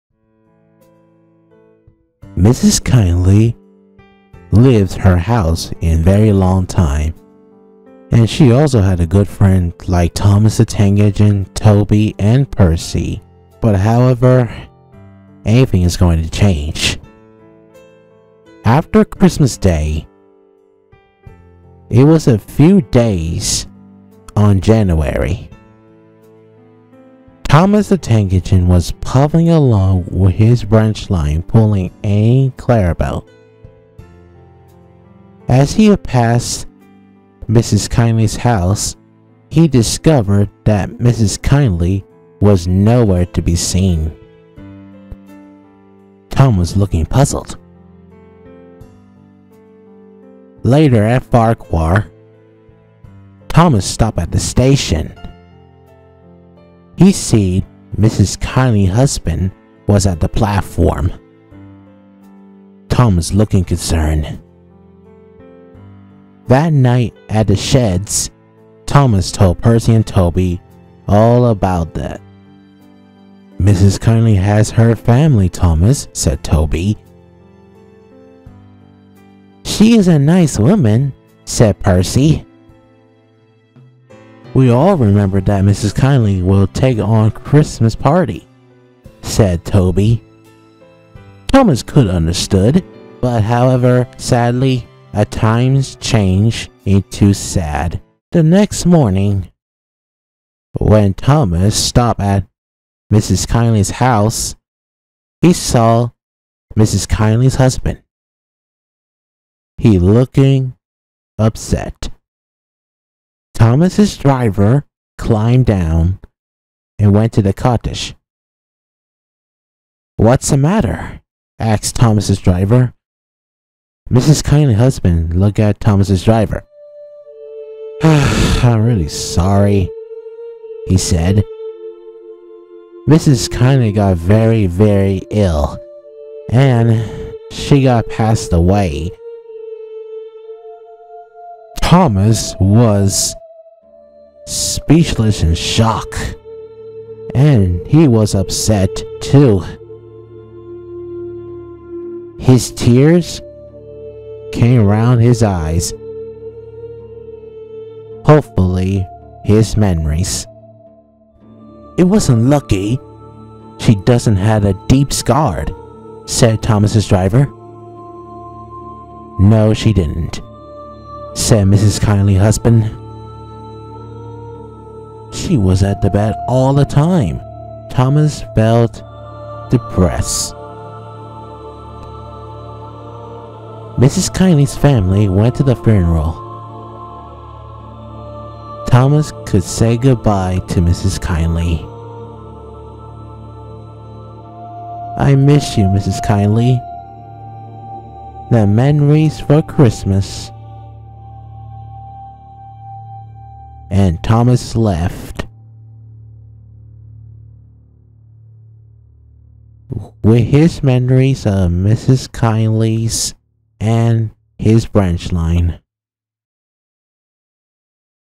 Deep Scar
Here's the twenty my written adaptations of Trainz Thomas and Friends.

NOTE: Instead of video ,I use audio and picture like a audiobook and my voice was very first attempting but grammar errors.